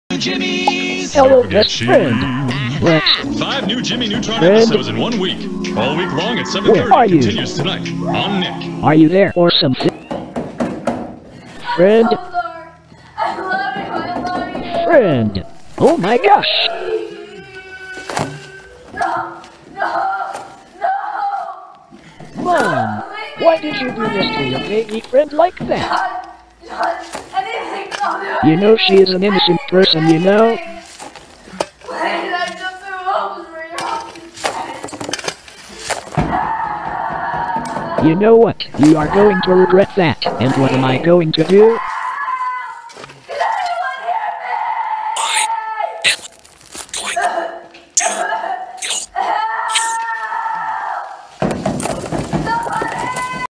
Kinito’s Reaction When Mom locks her friend out
KinitoPet is Mad at Friend’s Mommy

This is Really Bad :(